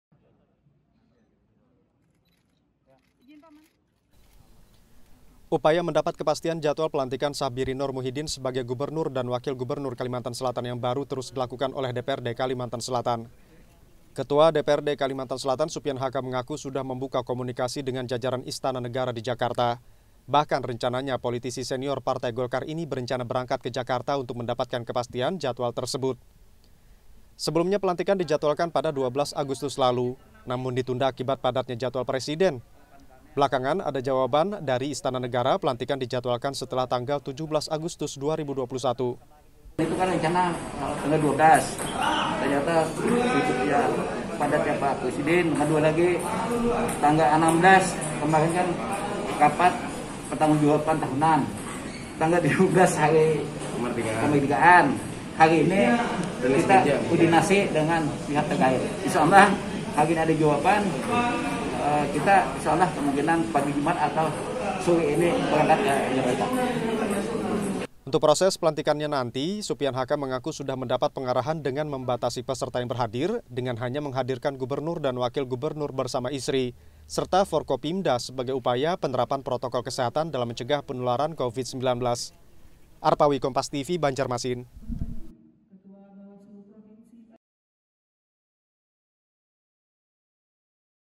DPRD Kalsel Upayakan Dapat Kepastian Pelantikan Gubernur dan Wakil Gubernur Kalsel Terpilih
BANJARMASIN, KOMPAS.TV - Upaya mendapat kepastian jadwal pelantikan sahbirin noor - muhidin sebagai gubernur dan wakil gubernur kalsel yang baru, terus dilakukan oleh dprd kalsel.

Ketua dprd kalsel, supian hk mengaku sudah membuka komunikasi dengan jajaran istana negara di jakarta.
Bahkan rencananya, politisi senior partai golkar tersebut akan berangkat ke Jakarta untuk mendapatkan kepastian jadwal tersebut.

Sebelumnya pelantikan dijadwalkan pada 12 agustus lalu.
Namun ditunda akibat padatnya jadwal presiden.
Belakangan, ada jawaban dari istana negara, pelantikan dijadwalkan setelah tanggal 17 agustus 2021.